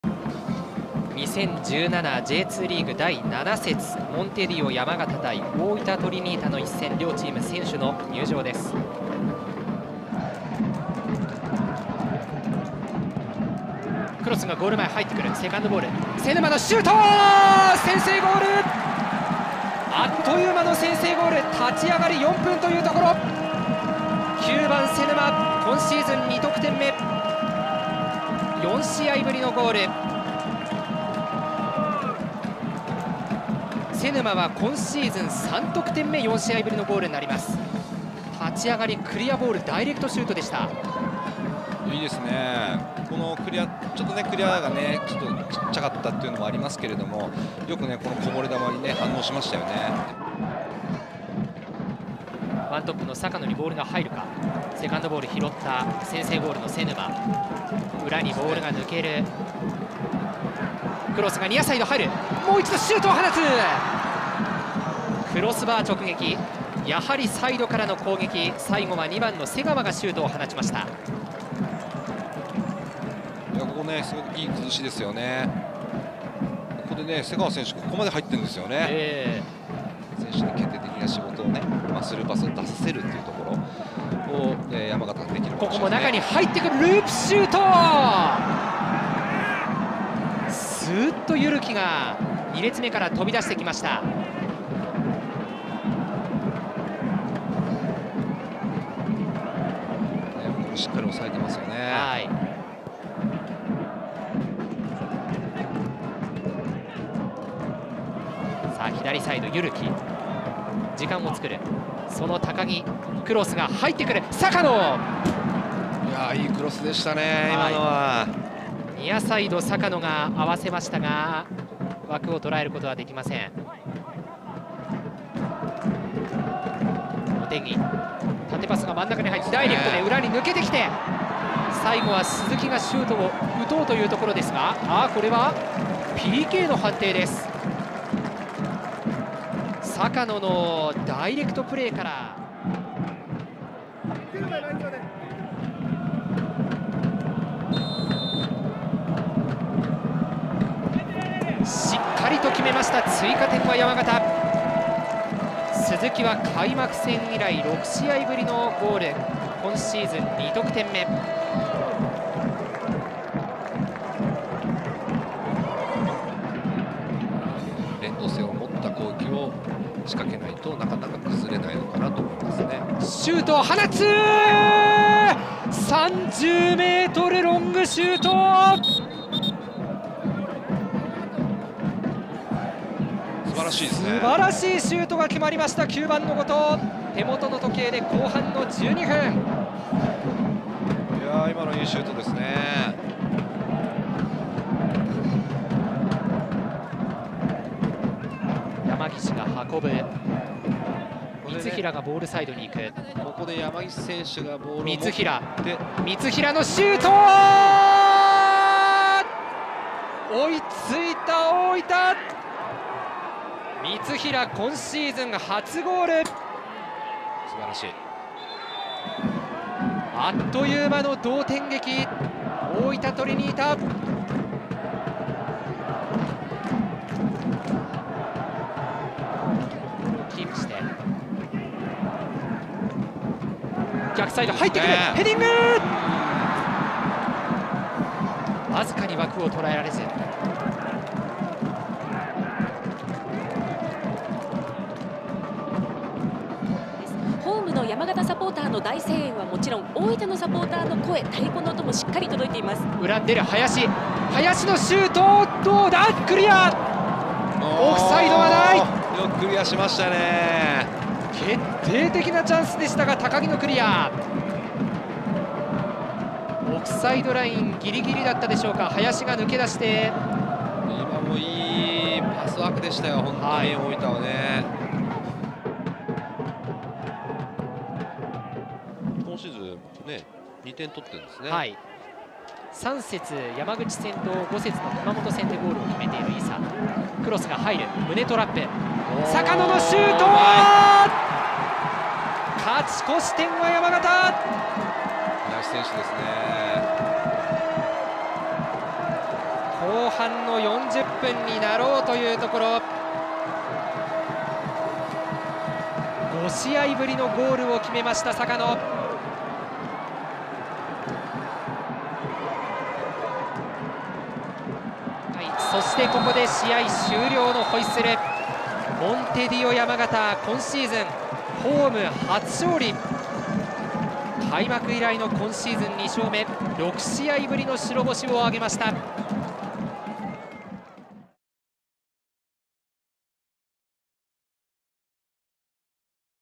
【ハイライト】2017明治安田生命J2リーグ第7節 モンテディオ山形 vs 大分トリニータ
2017明治安田生命J2リーグ第7節 vs モンテディオ山形戦のハイライト！